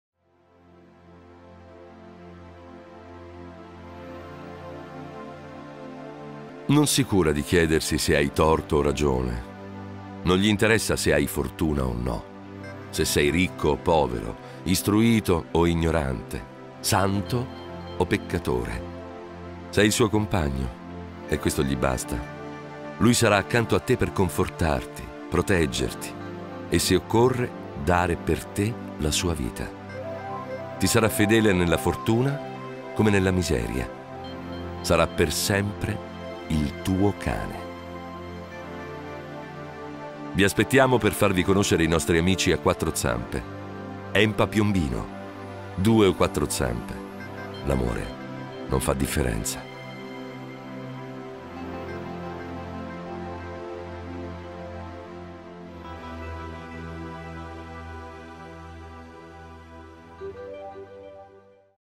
Campagna Adozioni ENPA 2017
La voce di Roberto Pedicini, vincitore di numerosi premi  per il suo doppiaggio di attori importanti come Jim Carrey in "The Truman Show" e di Kenneth Branagh in "Celebrity", per la campagna adozioni dei cani del canile municipale di Piombino